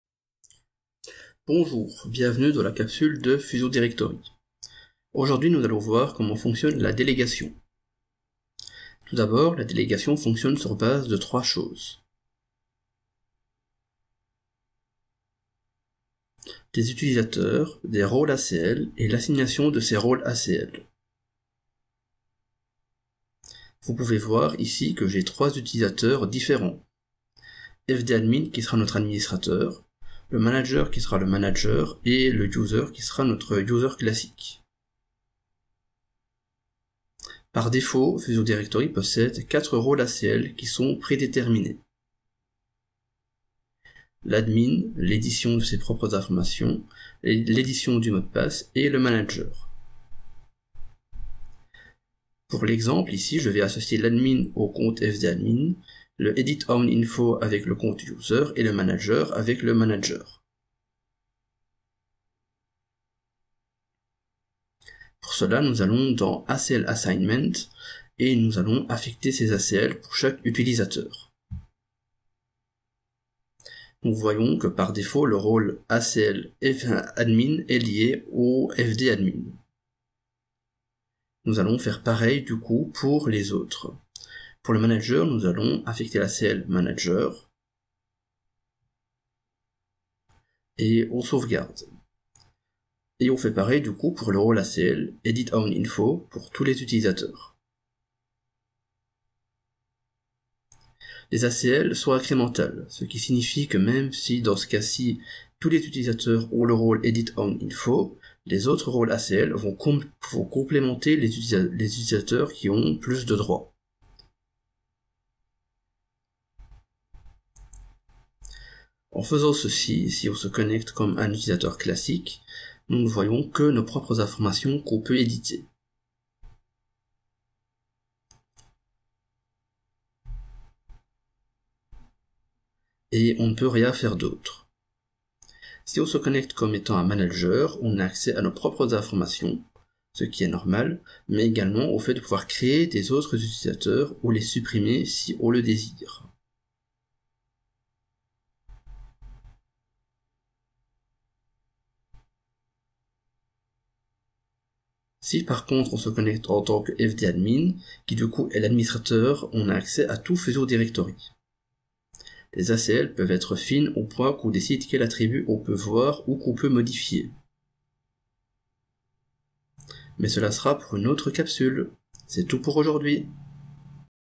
FusionDirectory Capsule 02 La délégation
Découvrez la gestion de la délégation dans FusionDirectory. Un des points forts de FusionDirectory est la possibilité de déléguer à d'autres personnes la gestion d'identités numériques dans FusionDirectory.

Ceci peut prendre de nombreuses formes, accès en lecture seule, accès en écriture a seulement certaines données etc.